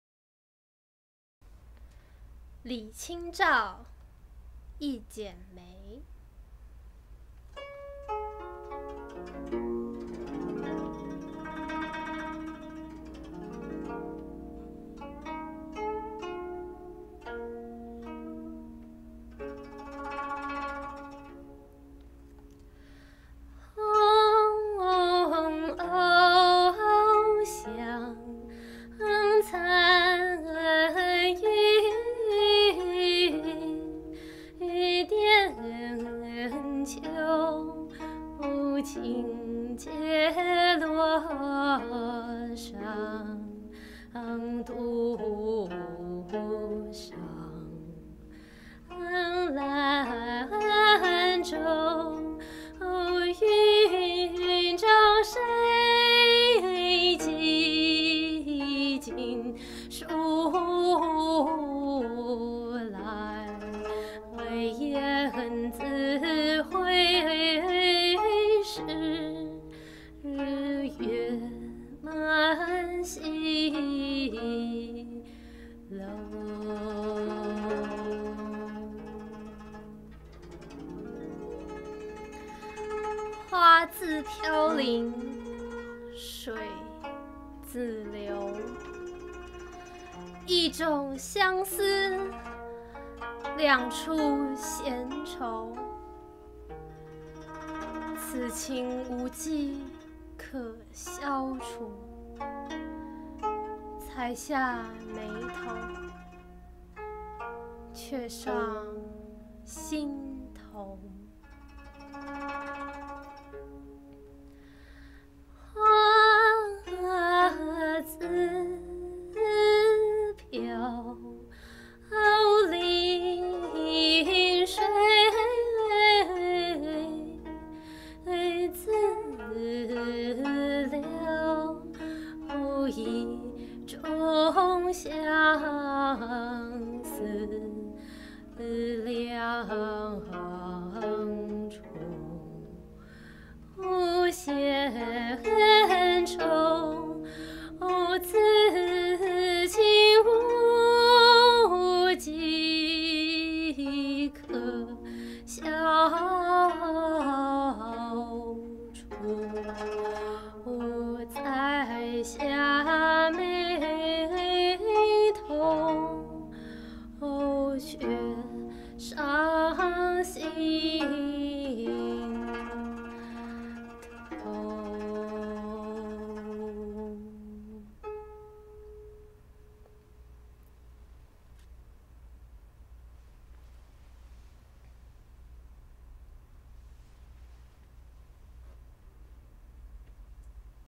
一剪梅
李清照〈一翦梅〉
紅藕香殘玉簟秋，輕解羅裳，獨上蘭舟。雲中誰寄錦書來，雁字回時，月滿西樓。
花自飄零水自流，一種相思，兩處閒愁。此情無計可消除，才下眉頭，卻上心頭